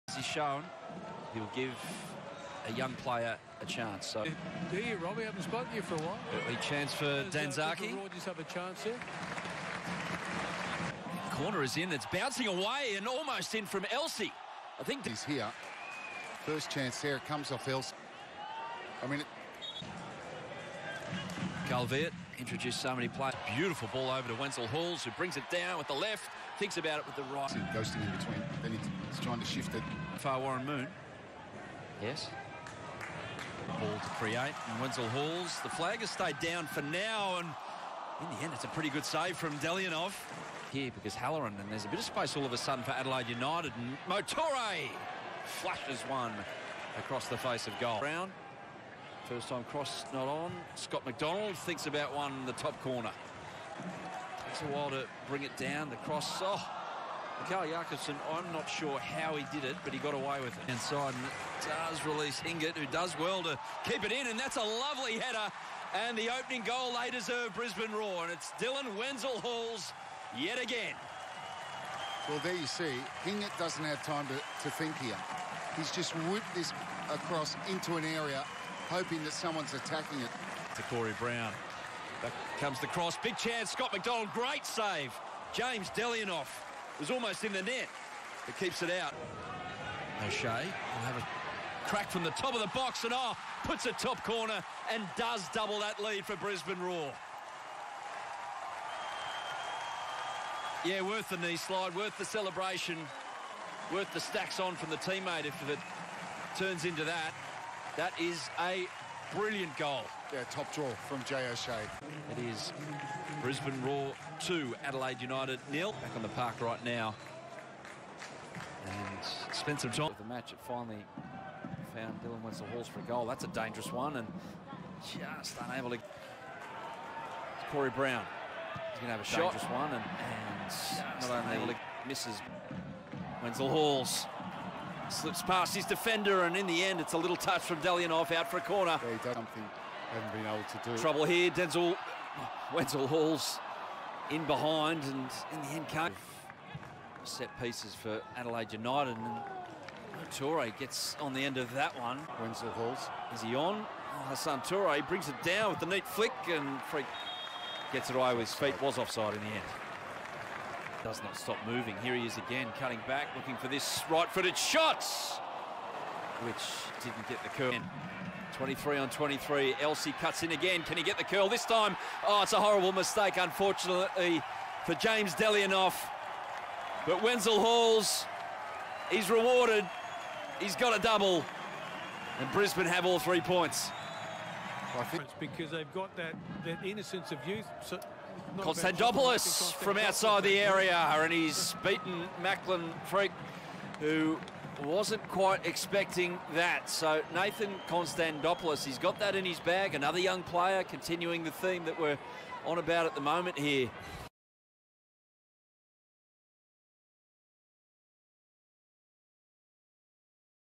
Brisbane Roar vs Adelaide United Highlights All Goals Hyundai A-League 30.01.2021
Brisbane Roar vs Adelaide United 3-1 Highlights All Goals Hyundai A-League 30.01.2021

Dylan Wenzel-Halls 37'
James O'Shea 45'
Dylan Wenzel-Halls 78'
Nathan Konstandopoulos 82'